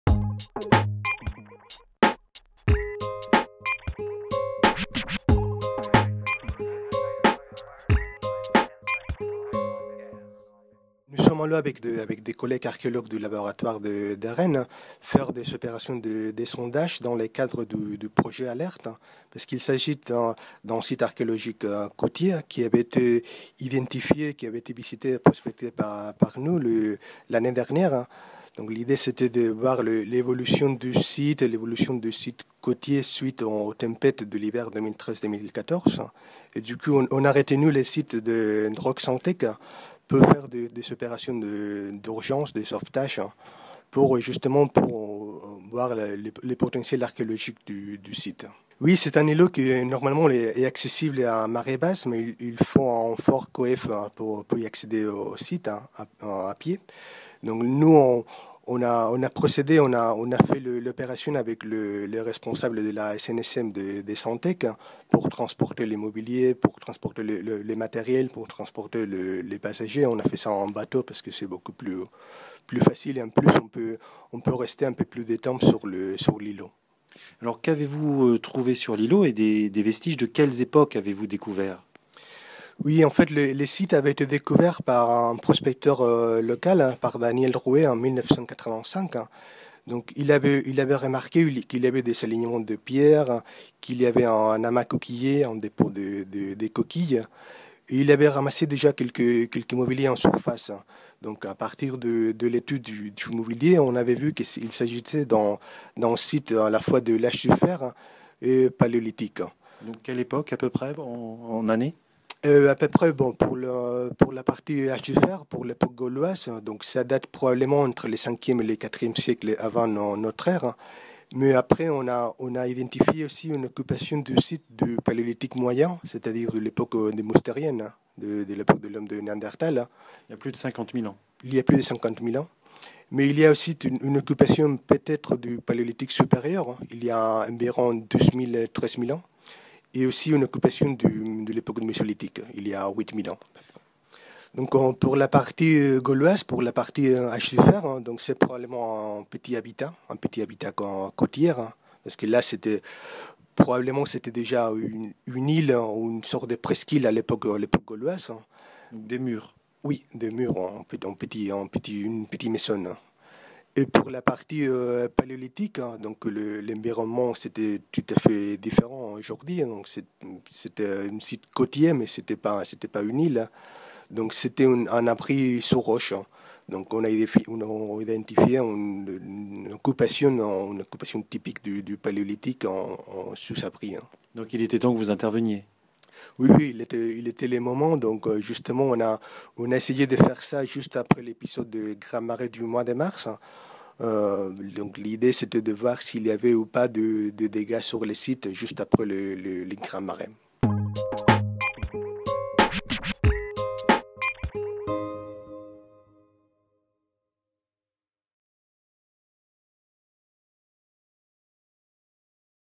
Entre l'archéologue et la mer, c'est la course !
Les archéologues creusent la terre. L'océan lui aussi, tempête après tempête, creuse la terre. Et parfois, il détruit des sites archéologiques ! En Bretagne, le projet Alert (Archéologie littorale et réchauffement terrestre) réunit des archéologues, des géographes et des géomorphologues pour intervenir in extremis sur des sites fragiles. En mars 2015, des chercheurs et des bénévoles ont mené une fouille de sauvetage sur l'îlot de Roc'h Santec, près de l'île de Batz (Finistère). Les explications de Pau Olmos Benloch, responsable du projet au laboratoire Creaah (Université de Rennes 1, CNRS). Interview Nicolas Guillas. Photos Meritxell Monros et François Le Gall. Montage Jacky Vignon. Production Espace des sciences.